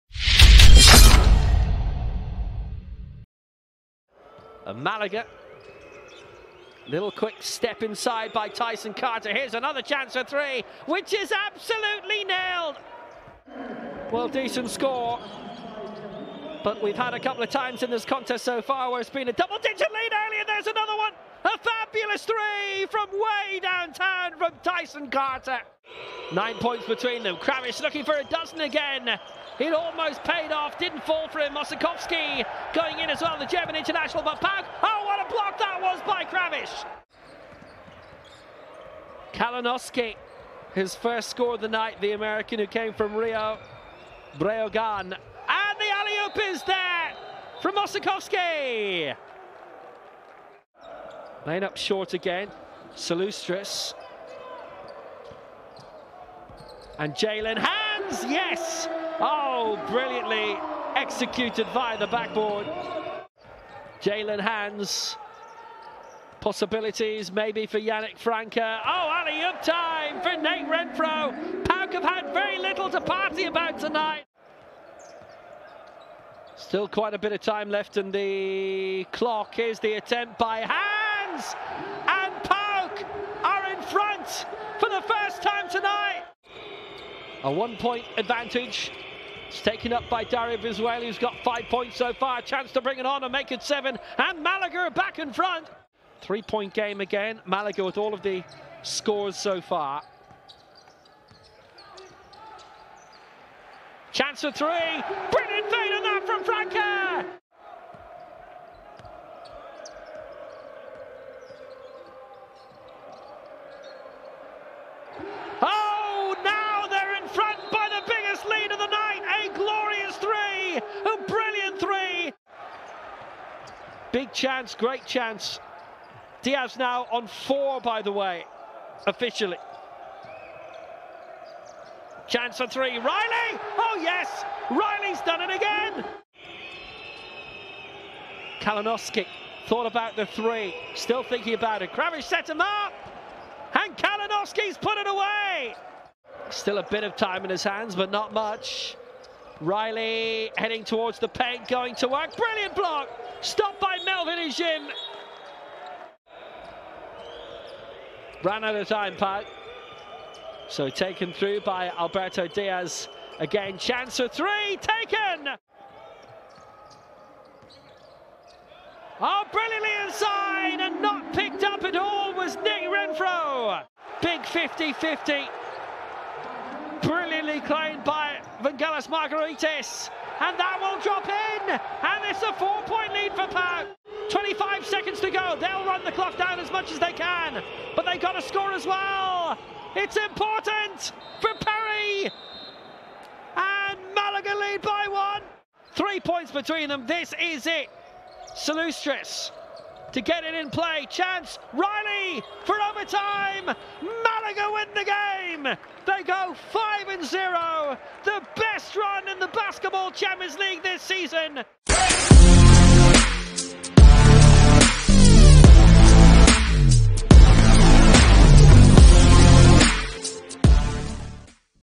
PAOK mateco v Unicaja | Week 9 | Highlights - Basketball Champions League 2022/23
Watch the highlights of the game between PAOK mateco and Unicaja from Week 9 of the FIBA Basketball Champions League 2022/23.